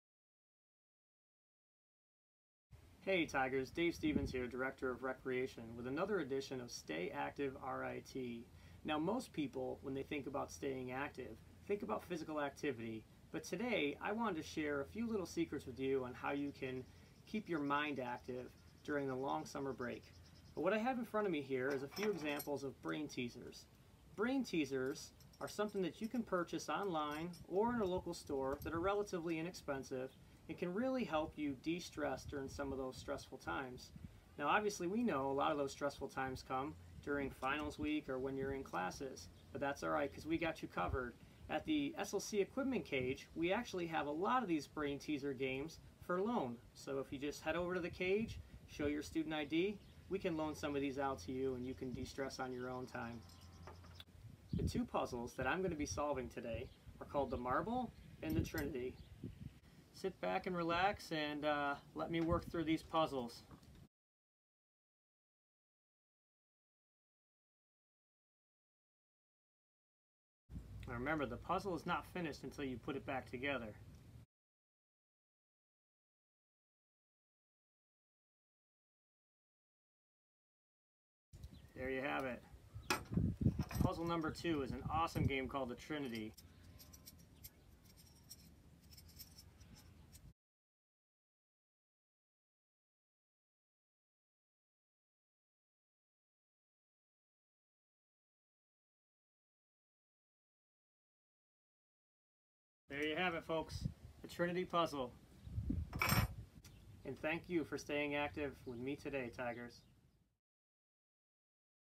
Stay Active RIT: Brain Teasers
Physical activity isn't the only way to stay active this summer. Join Dave Stevens, Director of Recreation at RIT, for some brain teaser ideas to keep your mind moving.